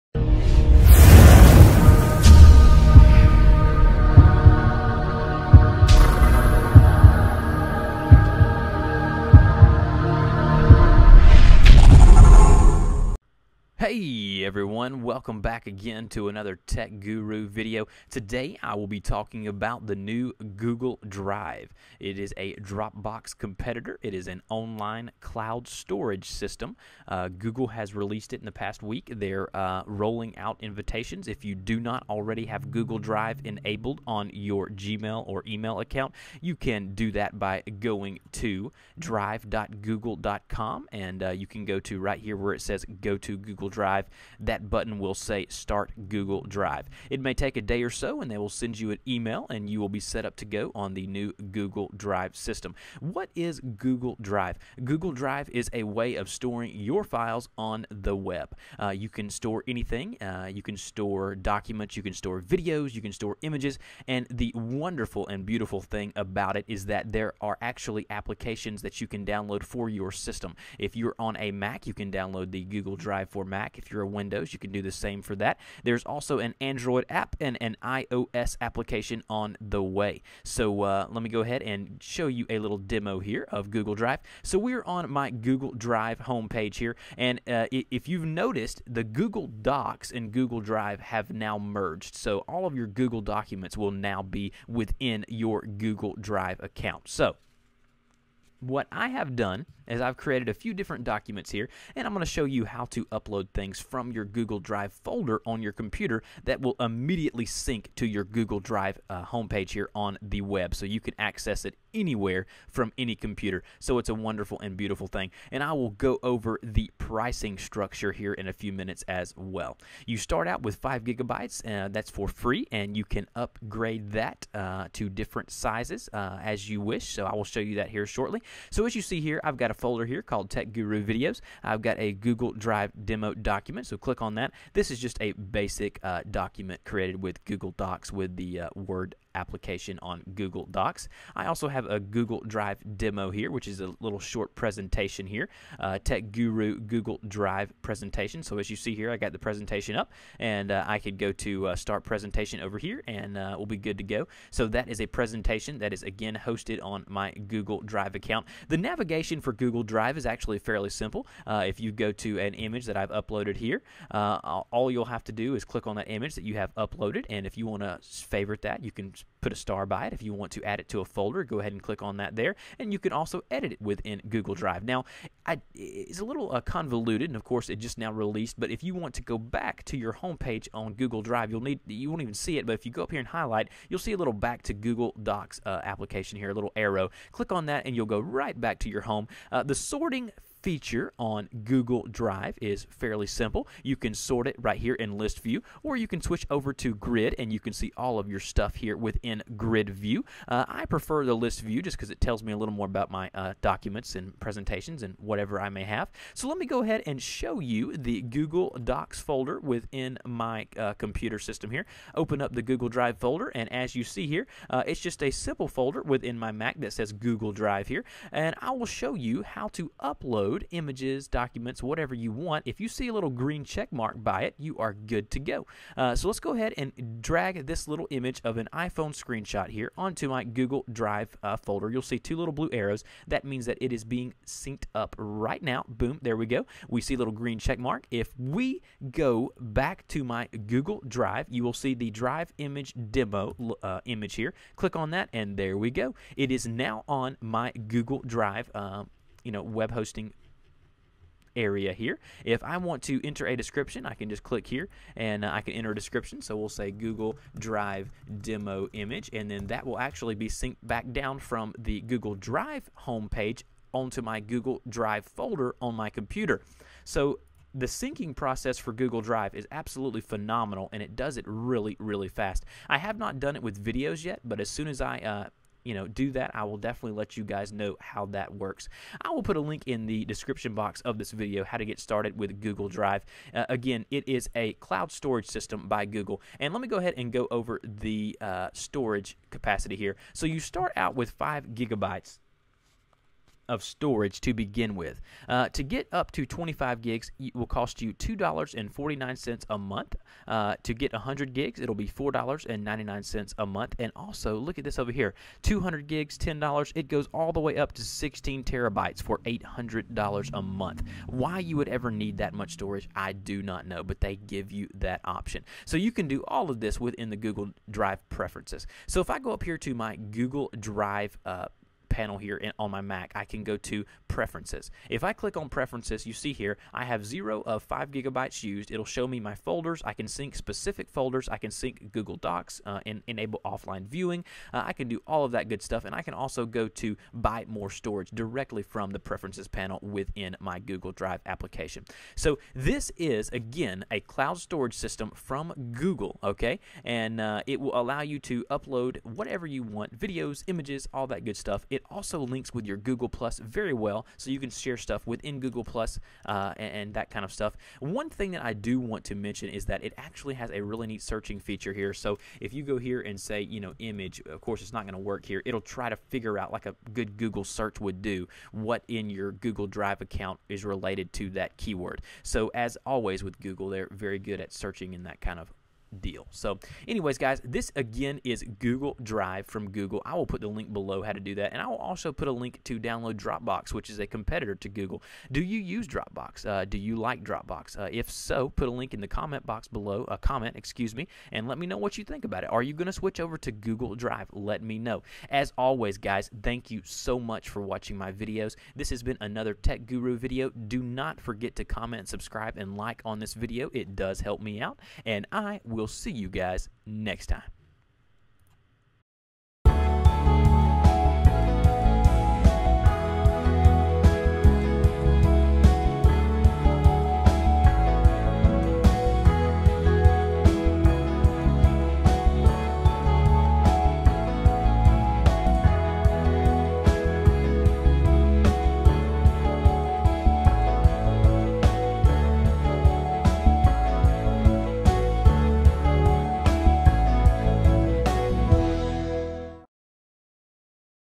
Google Launces Google Drive - Cloud Based Storage
In this video I go over the new Google cloud storage system "Google Drive".